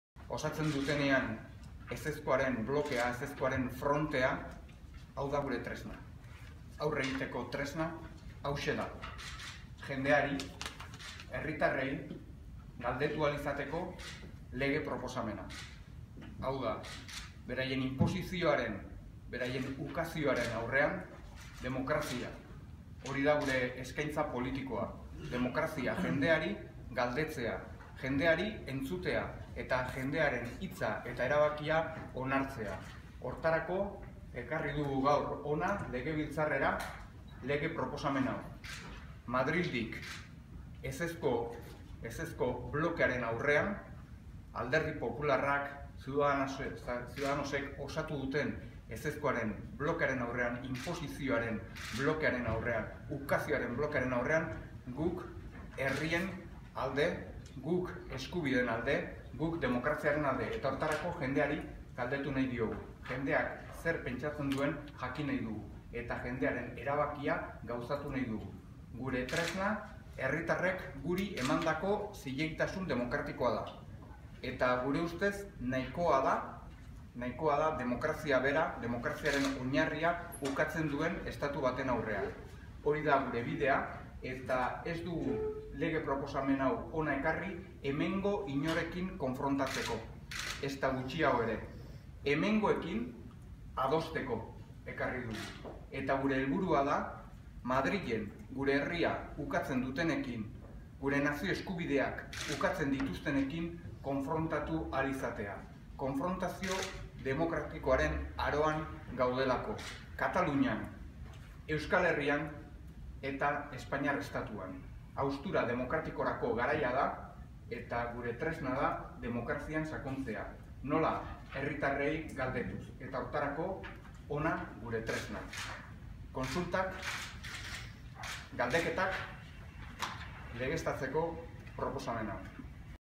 Ezezkoaren blokeari aurre egiteko tresna, herritarrei galdetu ahal izateko legea
Gaur aurkeztu dugu gure proposamena. Euskal herritarren artean adosteko, Madrilekin demokratikoki konfrontatzeko.